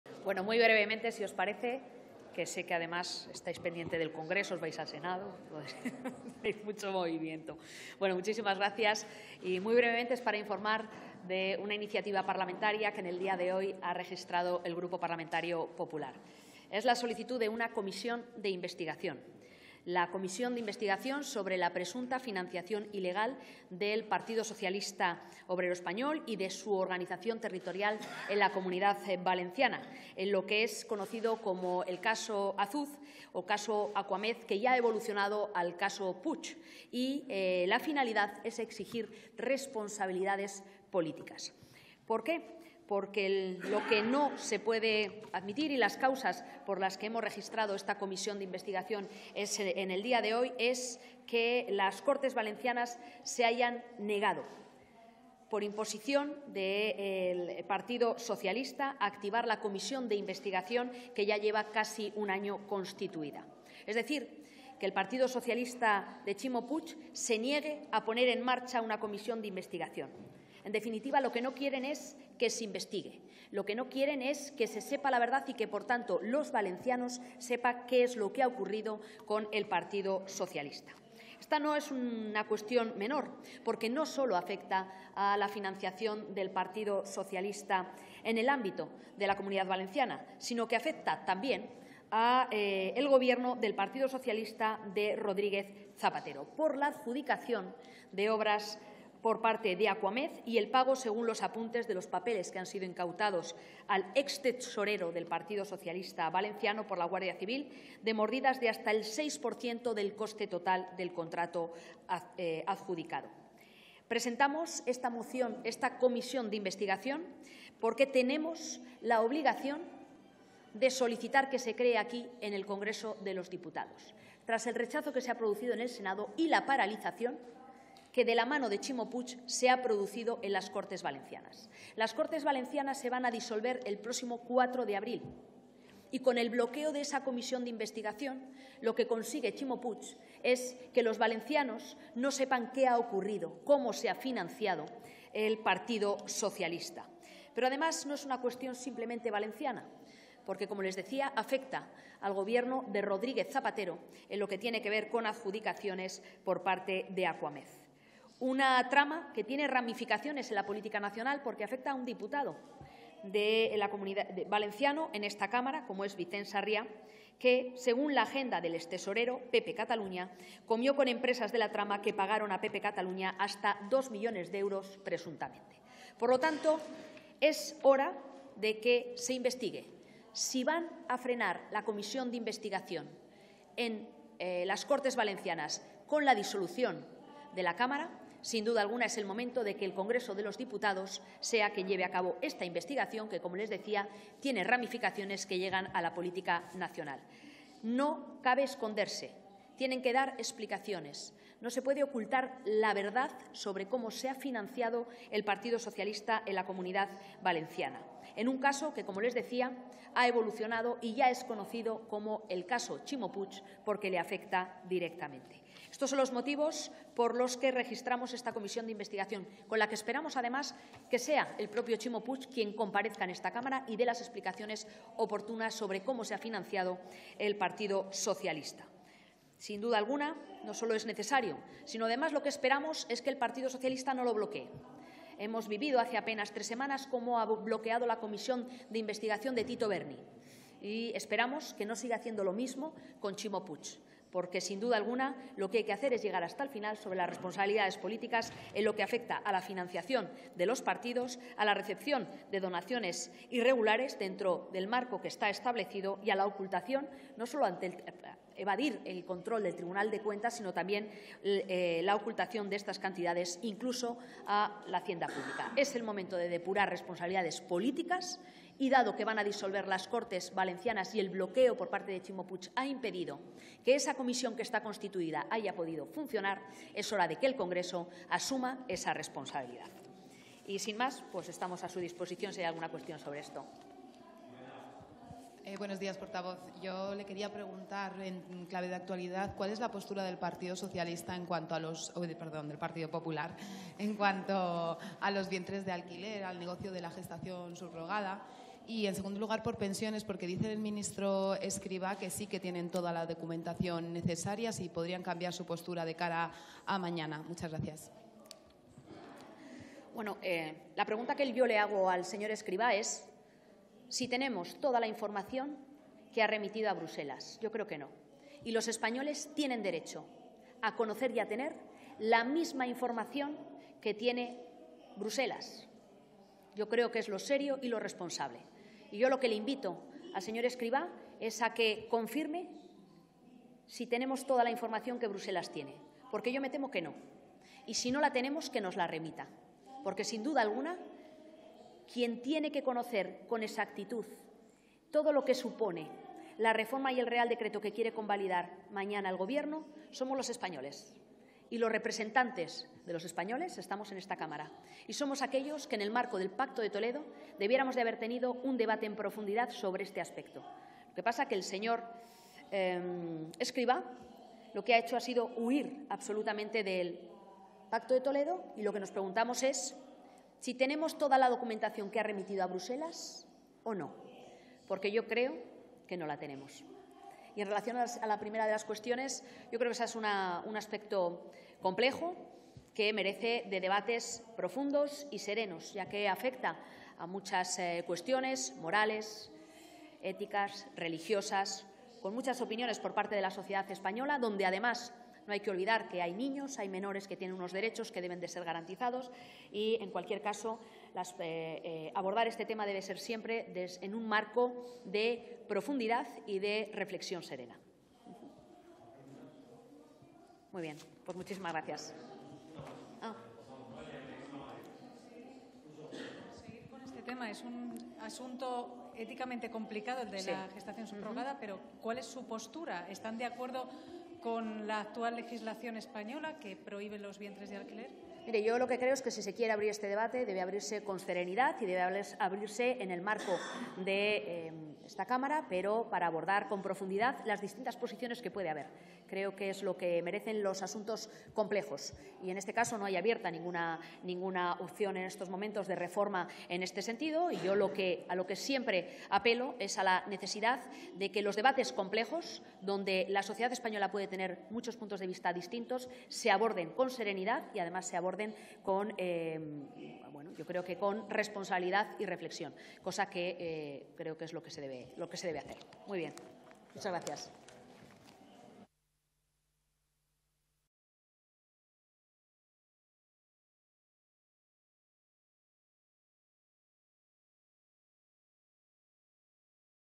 Gamarra solicita una comisión de investigación por el caso Azud en la Comunidad Valenciana
La portavoz del PP en el Congreso, Cuca Gamarra, ha anunciado esta iniciativa parlamentaria ante la disolución de las Cortes Valencianas, que impide poner en marcha la comisión de investigación constituida en el Parlamento regional, y después de que la Mesa del Senado rechazase el intento de los populares de investigar en la Cámara Alta.